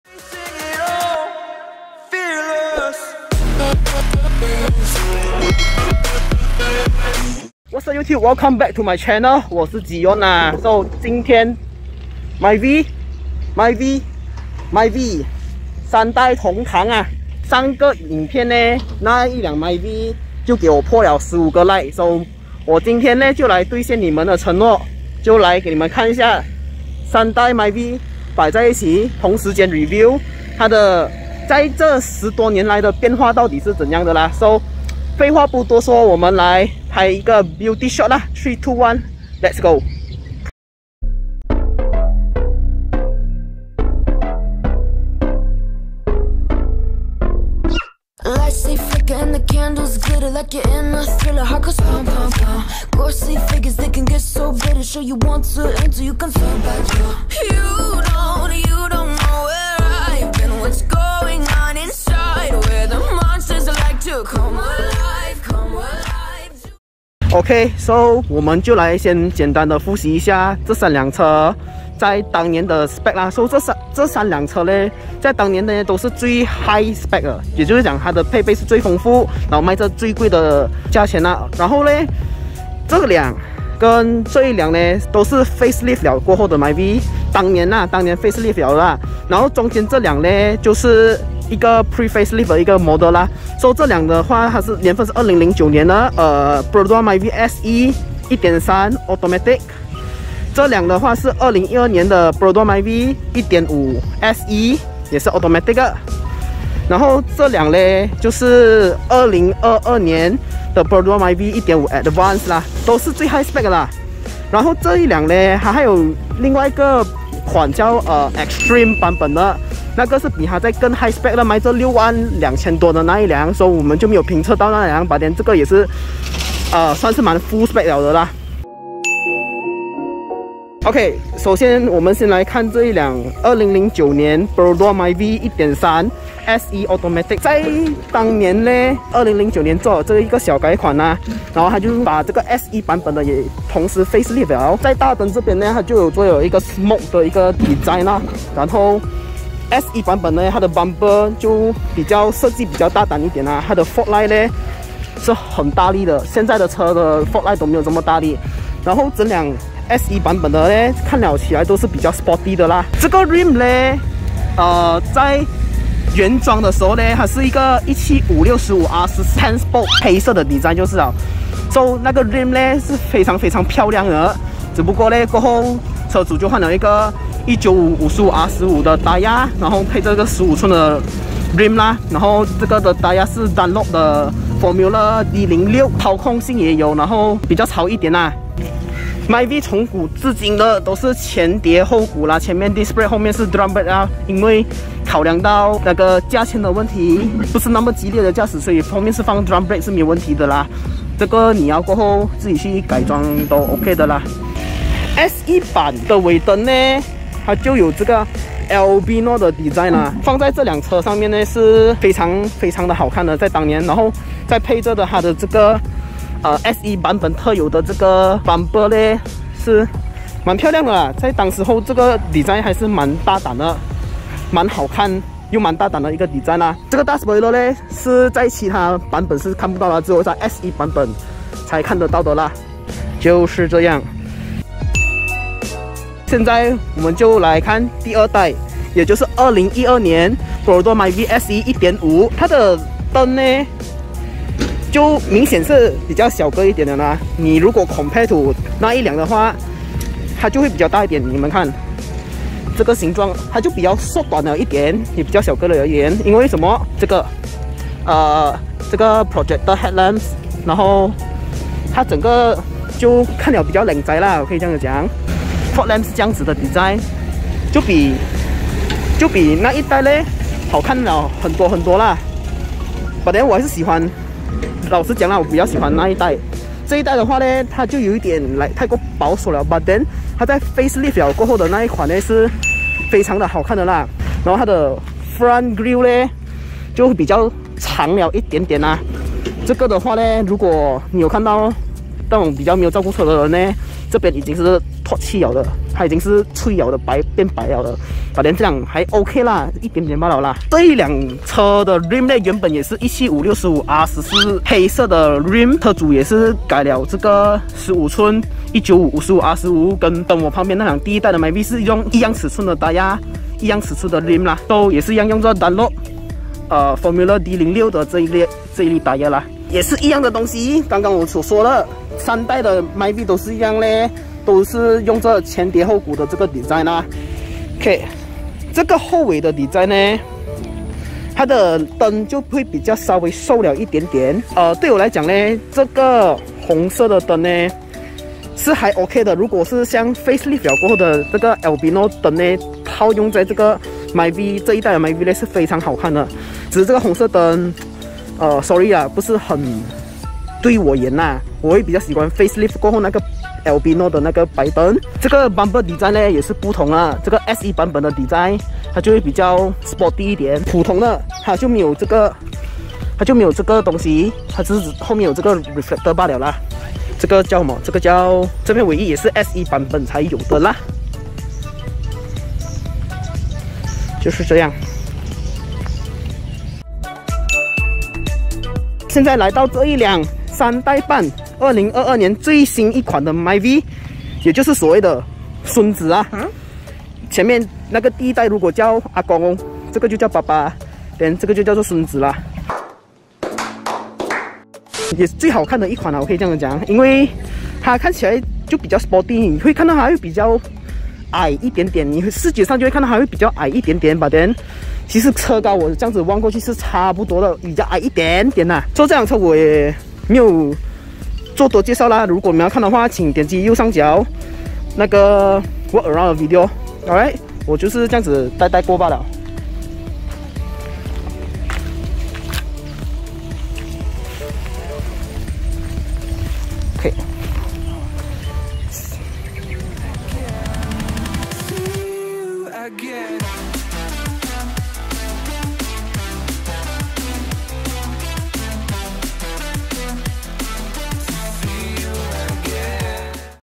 【Car Talk】#3 (Part 1) 3 Generation Perodua Myvisss under one tree ! (CC Subtitle)
Special thanks to the owners for lending me their car!